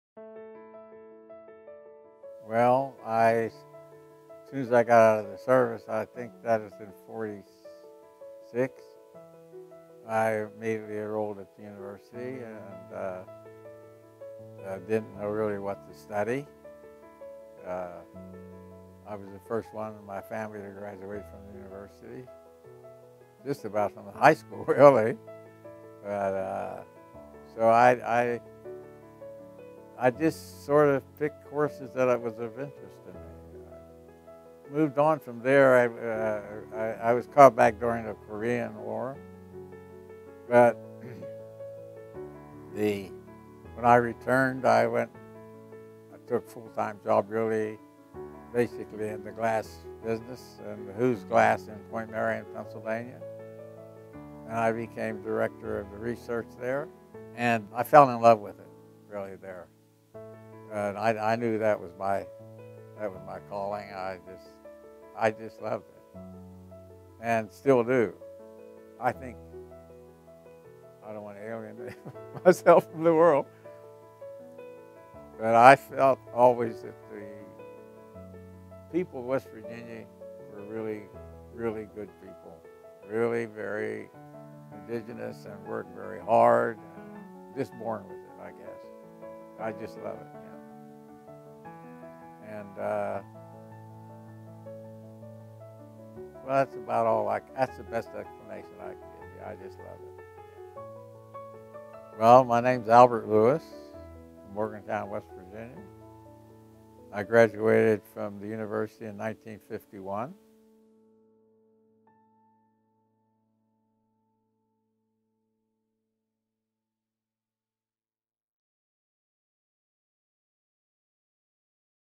WVU Alumni Spotlight | Albert Lewis (Glass Inc.)
Albert Lewis, a native of Morgantown, received his business degree in 1951 from West Virginia University. After serving in the United States Marine Corps, Lewis used his G.I. benefits to obtain his Business Degree. He also received a commission as an officer in the U.S. Army Signal Corps during the Korean conflict.

After military service, Lewis held several business management positions in the Morgantown and Pittsburgh areas. In 1956, he joined the Houze Glass Company to expand its research and development of commercial products and military applications of glass technology through contracts with the Navy’s Office of Naval Research, as well as, several military and government projects. Through his investigations of glass fiber and resin science he advanced the knowledge of the use of these materials for structural and fire protection applications.

Lewis went on to launch a business and science career in the development and manufacturing of high-strength and high-temperature glass fiber products. In 1970, after moving fiberglass technology to California based Aerojet General, he acquired their fiberglass assets and founded his current company, Glass Inc. International. He currently serves as the owner and CEO.

Glass Inc. International licenses glass fiber technology and provides research, engineering and consulting services to the glass industry. Lewis is responsible for 30 glass manufacturing plants that have been installed throughout the world. The company also operates a precision casting foundry, supplying in-house company requirements, as well as supplies the general market with a broad range of metal castings. The fiberglass plants have provided the residents of these countries access to insulation products to improve their living environment.

To date, Lewis holds 23 U.S. patents in glass technology and chemistry, 13 patents pending, for high-temperature insulation in the United States and Internationally. His most recent development is a high-temperature glass fiber material to protect the lives of airline passengers and crew in the event of an aircraft fire.

In addition to his career dedication, Lewis has made a life-long commitment to finding a cure for his daughter’s rare neurological disease in hopes to alleviate suffering for not only her but, others who are afflicted. He established the Helen Lewis Research Foundation at the University of Southern California to fund research. In 2016 Lewis moved the foundation to the WVU Rockefeller Neuroscience Institute where significant progress has been achieved. In addition, Lewis’ current research and development focus is to provide the United States with an economically viable method to gasify municipal garbage with glass at minimal impact on the environment.